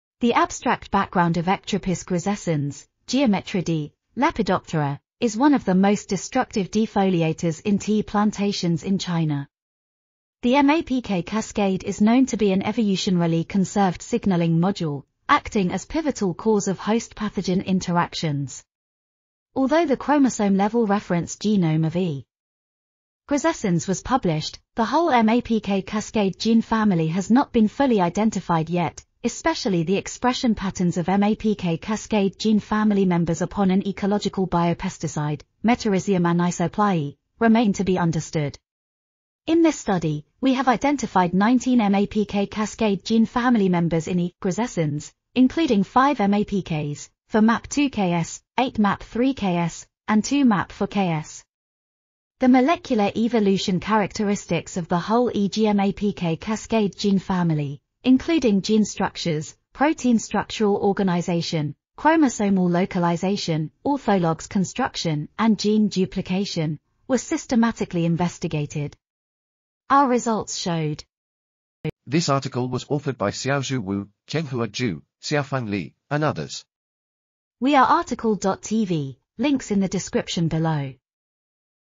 Genome-wide identification and immune response analysis of mitogen-activated protein ... | RTCL.TV
### Keywords ###

### Article Attribution ###
Title: Genome-wide identification and immune response analysis of mitogen-activated protein kinase cascades in tea geometrid, Ectropis grisescens Warren (Geometridae, Lepidoptera)
Authors: Xiaozhu Wu, Chenghua Zhou, Xiaofang Li, Jingyi Lin, Luis Carlos Ramos Aguila, Feng Wen ,and Liande Wang
Publisher: BMC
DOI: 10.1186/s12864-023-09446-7

### Video Timestamps ###
0:00:00 - Summary
0:01:11 - Title
0:01:17 - Outro
0:01:21 - End